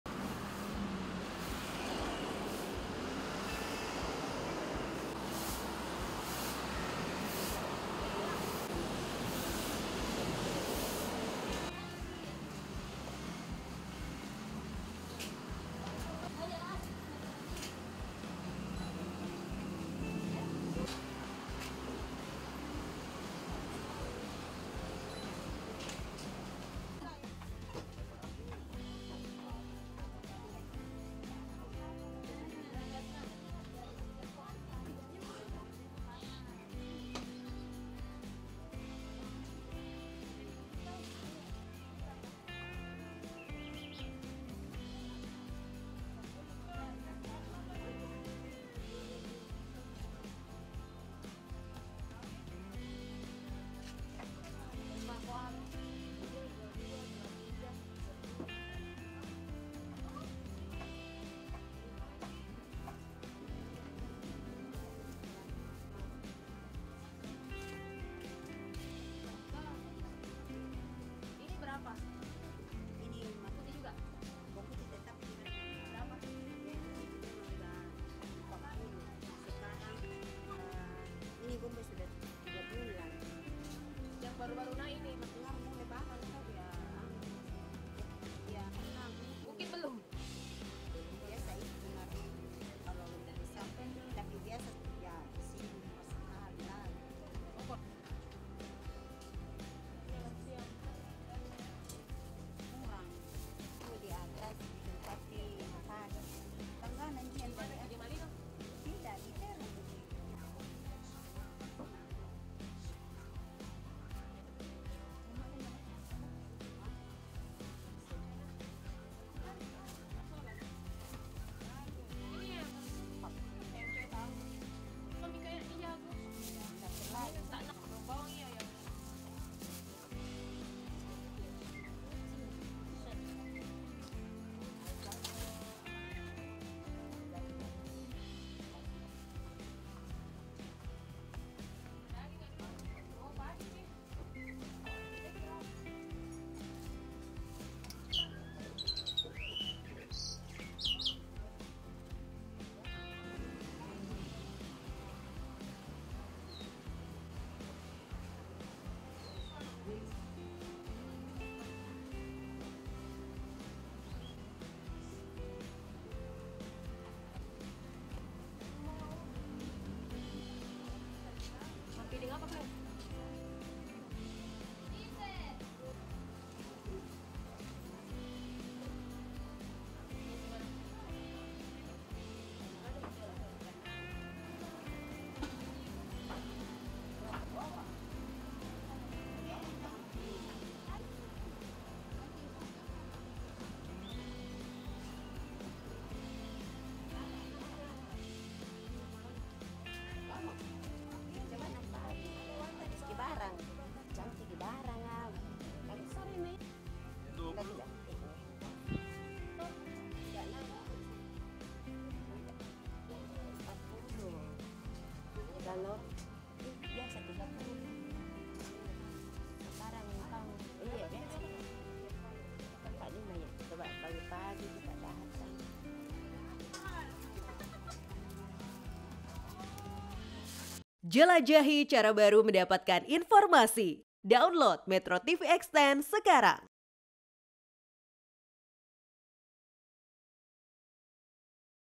Harga Bahan Pokok Naik Menjelang Idul Adha di Makassar
MetroTV, Harga Sejumlah Bahan Pokok Di Pasar Tradisional Di Kota Makassar Mulai Merangkak Naik Jelang Hari Raya Iduladha 1445 Hijriah Informasi Selengkapnya Akan Disampaikan Jurnalis Metro Tv Sabda Rolle Langsung Dari Makassar Sulawesi Selatan